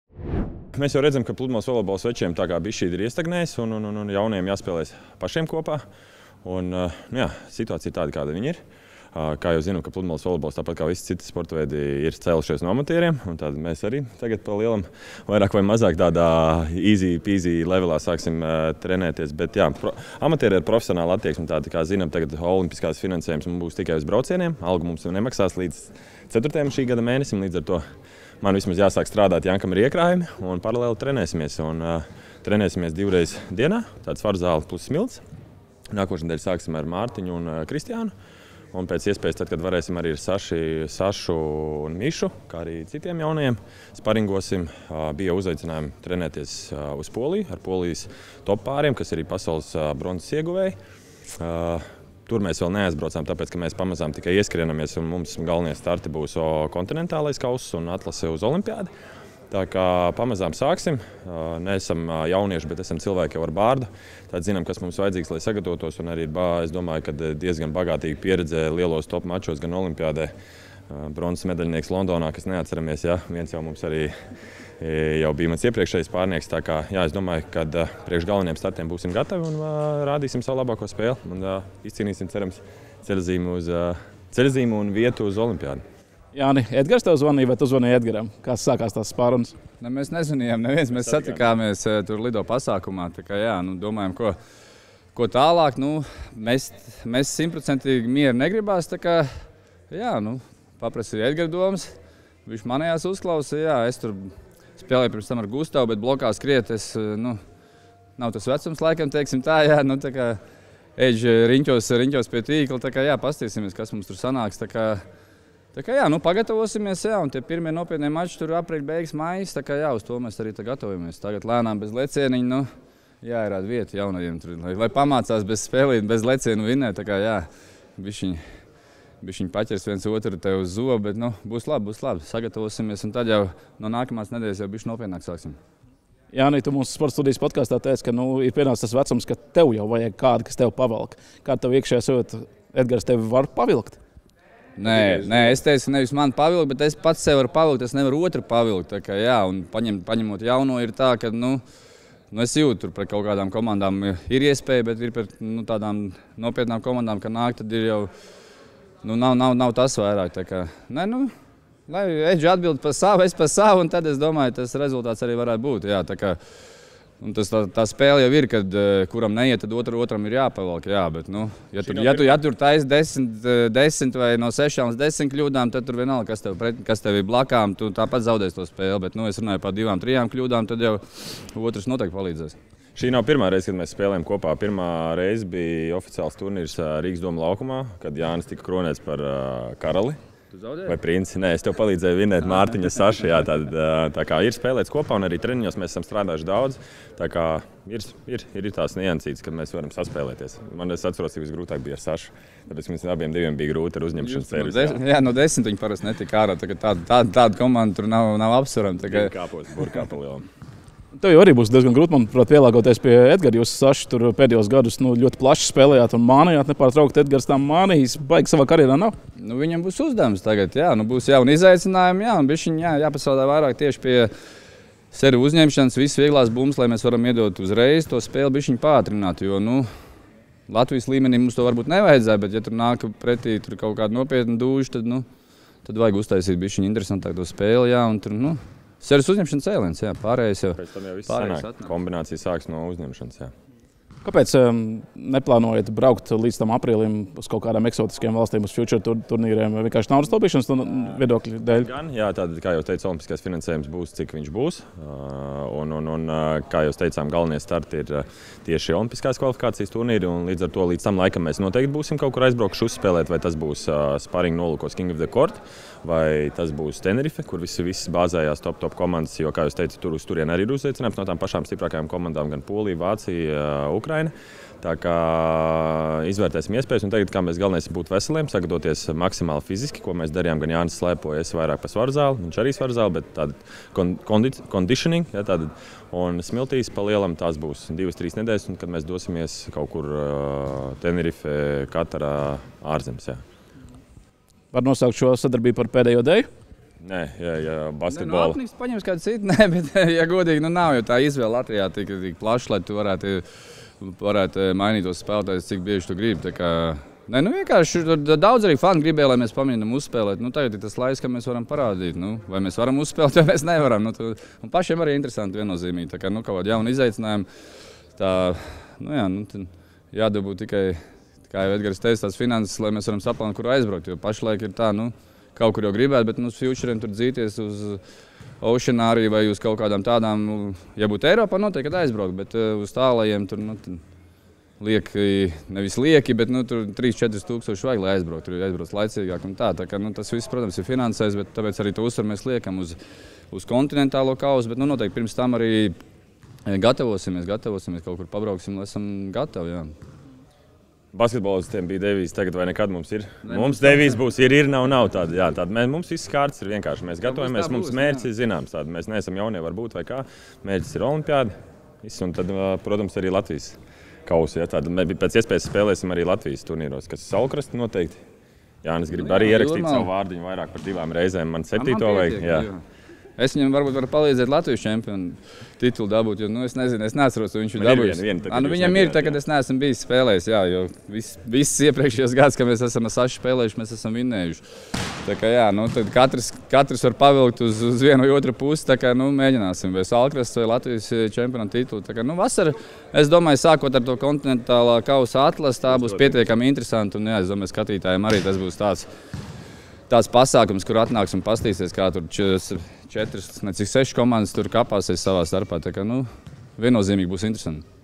JAUNS PLUDMALES VOLEJBOLA DUETS - Edgars Točs un Jānis Šmēdiņš! | Pludmales volejbols
Treniņus smiltīs sākuši Jānis Šmēdiņš un Edgars Točs. Pieredzējušies pludmales volejbolisti lēmuši visas kārtis likt un šo sezonu apvienojot spēkus vienā komandā, lai cīnītos par spēlēšanu Parīzes Olimpiskajās spēlēs šovasar.